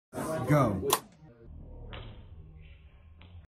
Horizontal Launch Drop Shoot Apparatus demo
A ball dropped from rest, and another ball launched horizontally, both at the same time and from the same height, will hit the ground at the same time (ignoring air resistance). The launched ball will travel forwards, but both balls accelerate downward at the same rate.